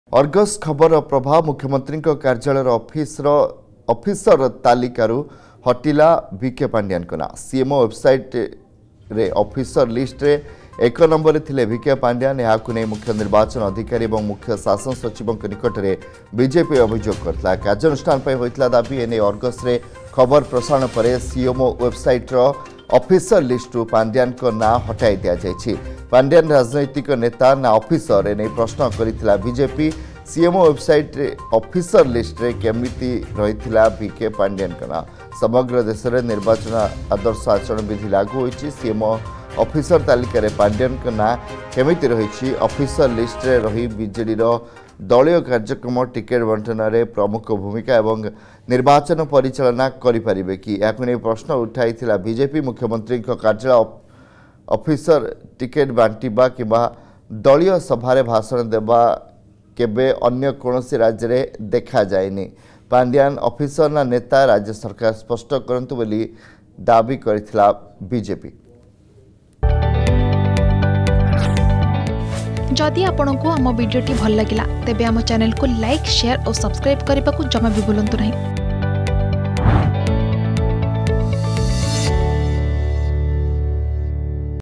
CMO ତାଲିକାରୁ ହଟିଲେ ପାଣ୍ଡିଆନ || VK Pandian
ଅର୍ଗସ୍ ଖବରର ପ୍ରଭାବ। ମୁଖ୍ୟମନ୍ତ୍ରୀଙ୍କ କାର୍ଯ୍ୟାଳୟର ଅଫିସର ତାଲିକାରୁ ହଟିଲେ ଭି. କେ ପାଣ୍ଡିଆନ। ସିଏମଓ ୱେବସାଇଟ ଅଫିସର ଲିଷ୍ଟରେ ଏକ ନମ୍ବରରେ ଥିଲେ ଭି.କେ. ପାଣ୍ଡିଆନ । ଏହାକୁ ନେଇ ମୁଖ୍ୟ ନିର୍ବାଚନ ଅଧିକାରୀ ଓ ମୁଖ୍ୟ ଶାସନ ସଚିବଙ୍କ ନିକଟରେ ଅଭିଯୋଗ କରିଥିଲା ବିଜେପି ।